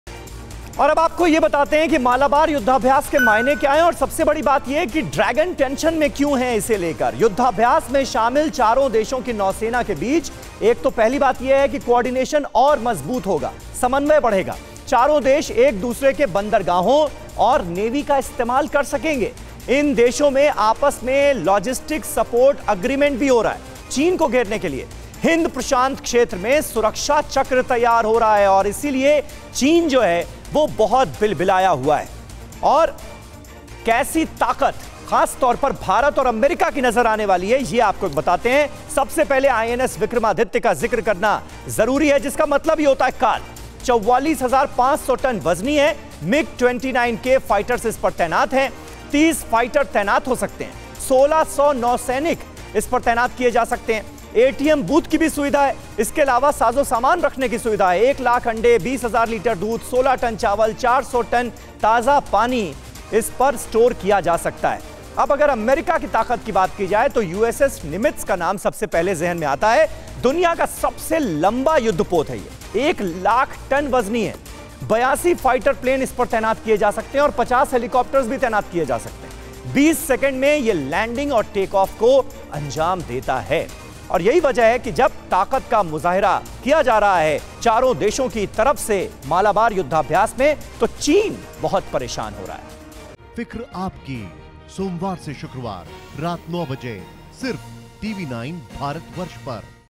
मालाबार से China को टेंशन, ग्राफिक्स से समझिए अमेरिकी नेवी की ताकत
मालाबार युद्धाभ्यास से चीन को टेंशन होने लगी है.अरब सागर में उतरी 4 देशों की सेनाओं ने चीन की नींद उड़ा दी है.